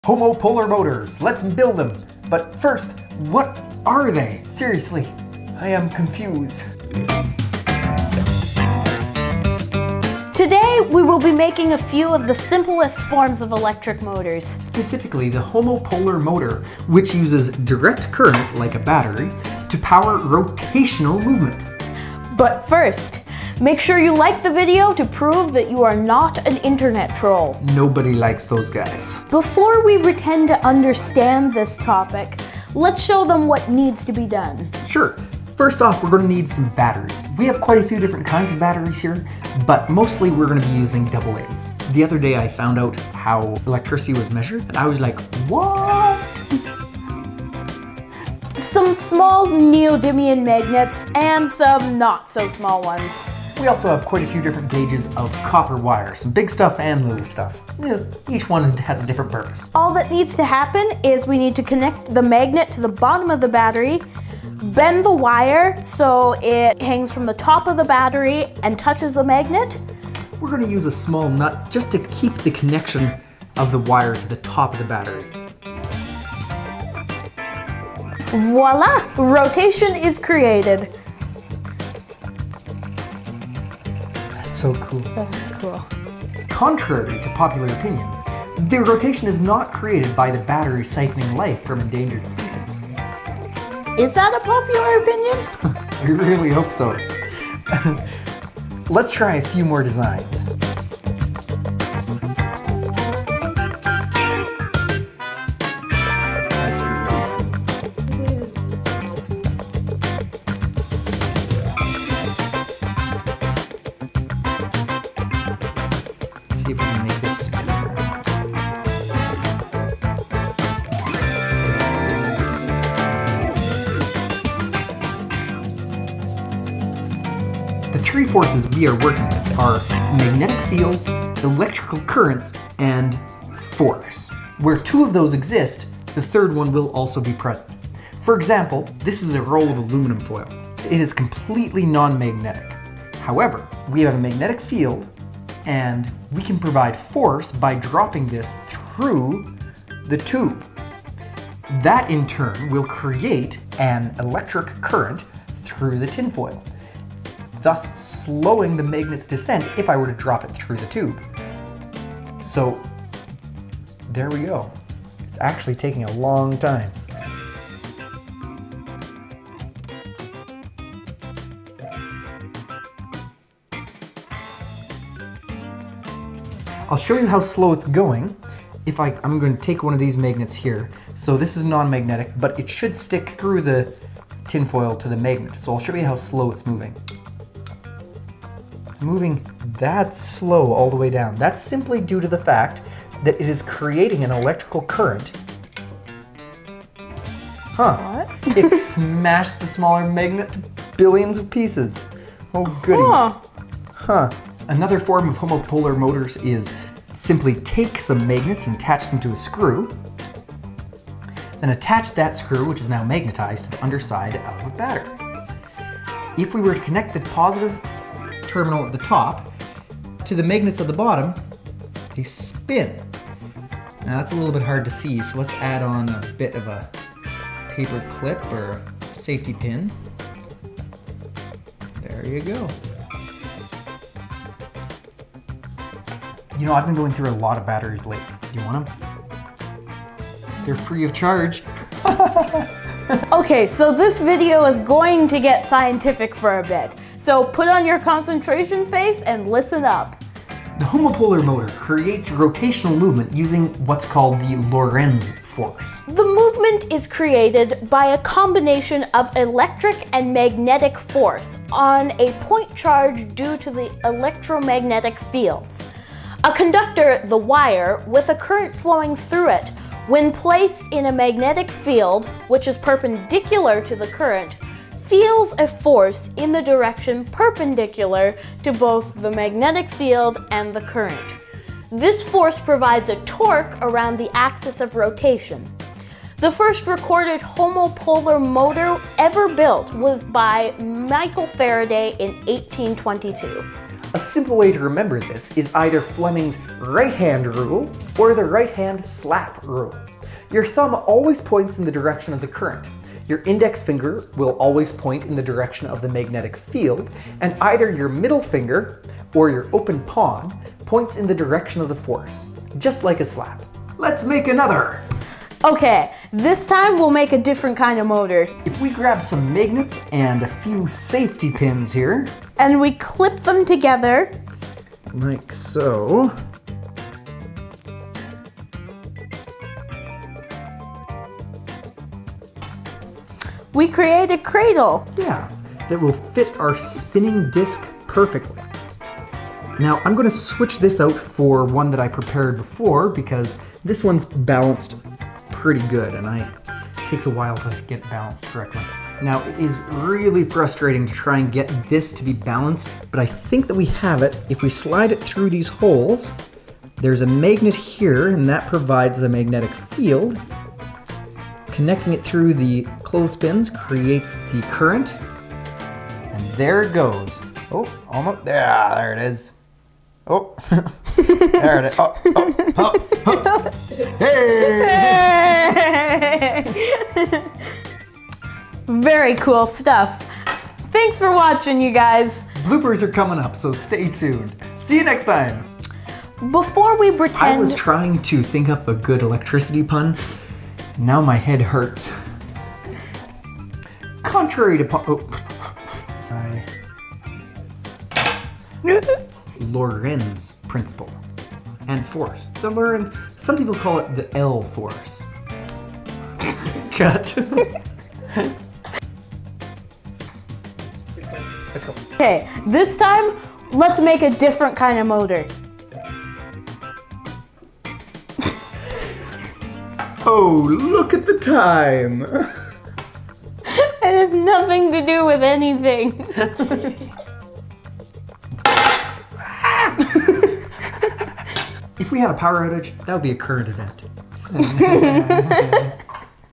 Homopolar motors - Fleming's right hand rule with the Lorentz force.
Let's make simple electric motors using the Lorentz force and Fleming's right hand rule! Specifically, a bunch of different types of homopolar motors. Join Destructive Creativity as we make bad puns, and break magnets in the pursuit of fun science!

The Lorentz force is responsible for the amazing phenomenon of homopolar motors. Stick a magnet to a battery and hang a copper wire off the top, and the wire start to spin! Easily remembered by Fleming's right hand rule, or the right hand slap rule, homopolar motors make for awesome science demonstrations or science fair projects! All you need to make these at home are some magnets, AA batteries, and some copper wire. The possibilities are endless!

We make 3 different kinds of homopolar motors, and and really cool experiment with aluminum foil and super magnets.We also do our best to explain exactly what is happening.

Destructive Creativity and Jonathan Allers are dedicated to providing fun and safe science experiments for all ages over 13 years old.